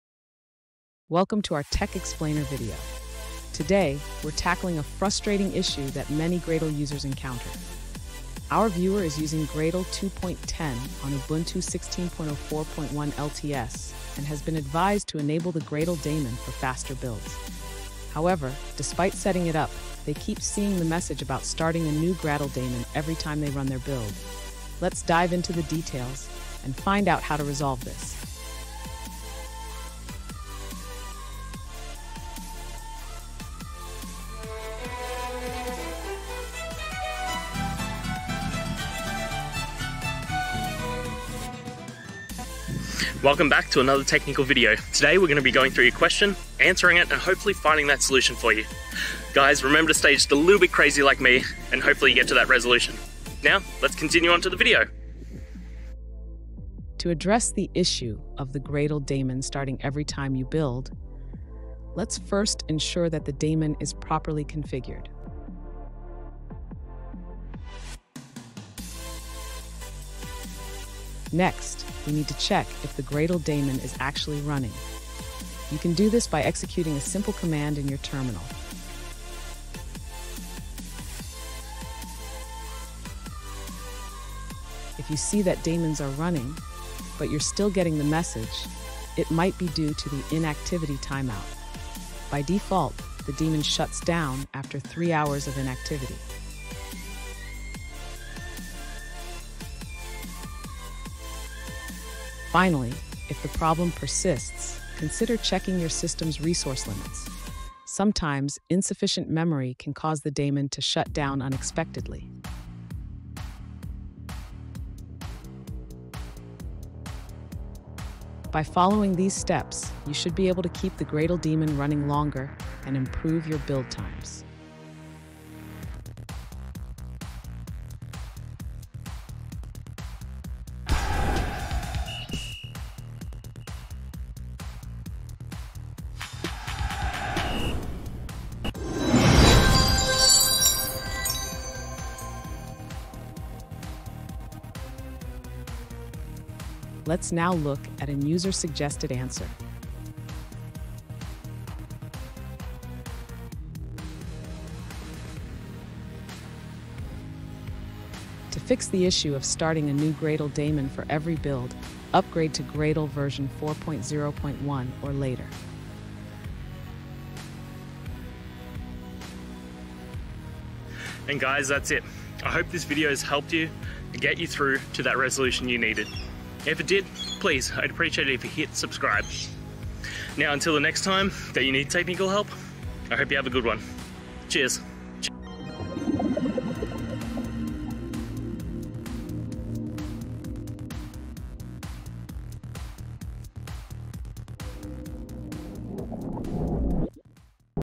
Fixing 'Starting a New Gradle Daemon' Warning for Faster Builds
In this video, we tackle a common issue faced by developers using Gradle: the 'Starting a New Gradle Daemon' warning. This warning can significantly slow down your build times, impacting your productivity. Join us as we explore effective strategies to resolve this warning and optimize your Gradle builds for a smoother and faster development experience.

Today's Topic: Fixing 'Starting a New Gradle Daemon' Warning for Faster Builds

Related to